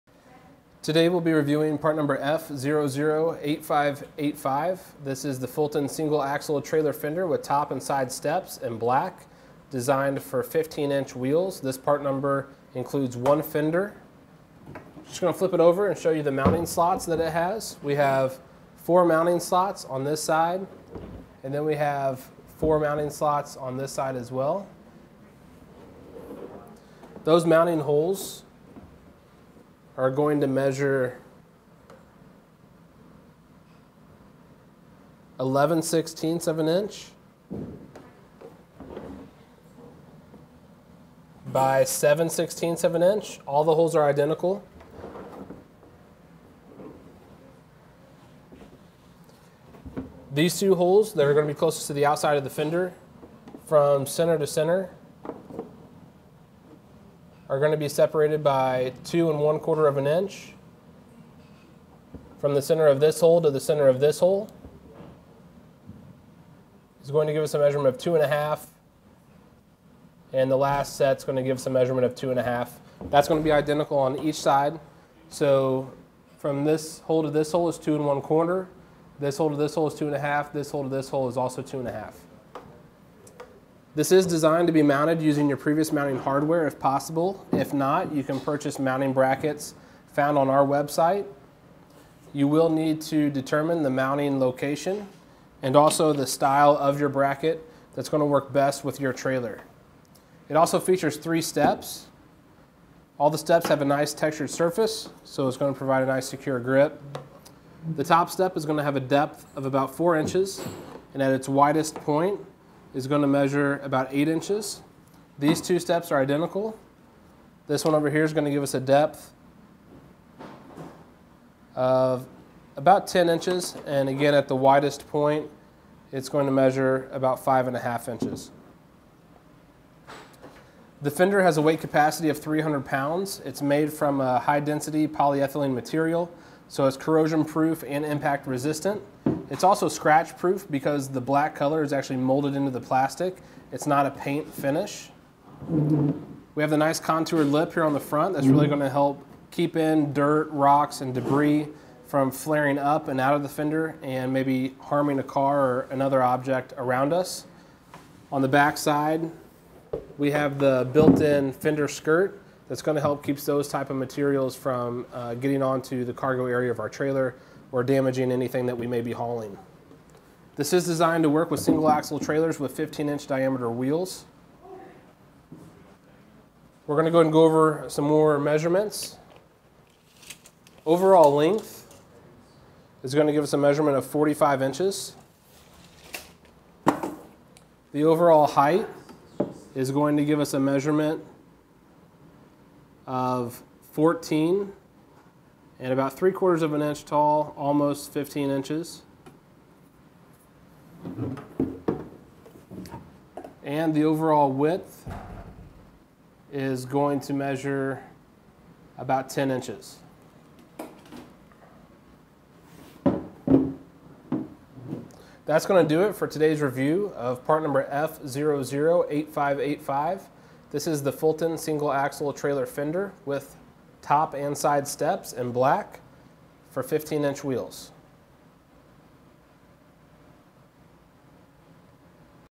etrailer | Fulton Single Axle Trailer Fender with Top and Side Steps Spec Review
Today we'll be reviewing part number F008585. This is the Fulton Single Axle Trailer Fender with top and side steps in black, designed for 15" wheels. This part number includes one fender. I'm just going to flip it over and show you the mounting slots that it has. We have four mounting slots on this side and then we have four mounting slots on this side as well. Those mounting holes are going to measure 11/16" x 7/16".

All the holes are identical. These two holes that are going to be closest to the outside of the fender, from center to center are going to be separated by 2 1/4". From the center of this hole to the center of this hole, is going to give us a measurement of 2 1/2" and the last set is going to give us a measurement of 2 1/2". That's going to be identical on each side. From this hole to this hole is 2 1/4".

This hole to this hole is 2 1/2". This hole to this hole is also 2 1/2". This is designed to be mounted using your previous mounting hardware, if possible. If not, you can purchase mounting brackets found on our website. You will need to determine the mounting location and also the style of your bracket that's going to work best with your trailer. It also features three steps.

All the steps have a nice, textured surface, so it's going to provide a nice, secure grip. The top step is going to have a depth of about 4" and at it's widest point it's going to measure about 8". These two steps are identical. This one over here is going to give us a depth of about 10" and at the widest point it's going to measure about 5 1/2". The fender has a weight capacity of 300 pounds. It's made from a high-density polyethylene material, so it's corrosion proof and impact resistant.

It's also scratch proof, because the black color is actually molded into the plastic. It's not a paint finish. We have the nice contoured lip around the front, that's really going to help keep in dirt, rocks, and debris from flaring up and out of the fender and maybe harming the car or another object around us. On the back side, we have the built in fender skirt that's going to help keep those types of materials from getting onto the cargo area of our trailer or damaging anything that we may be hauling. This is designed to work with single axle trailers with 15" diameter wheels. We're going to go over some more measurements. Overall length is going to give us a measurement of 45". Overall height is going to give us a measurement of about 14 3/4" tall, almost 15". The overall width is going to measure about 10". That's going to do it for today's review of part number F008585. This is the Fulton Single Axle Trailer Fender with top and side steps in black for 15" wheels. .